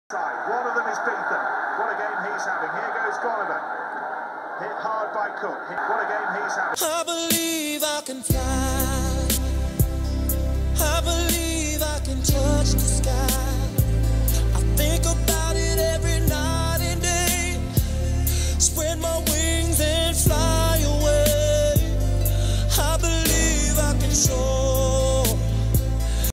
Flying John Afoa
Fomer All Black prop John Afoa takes flight while playing for Gloucester against Leicester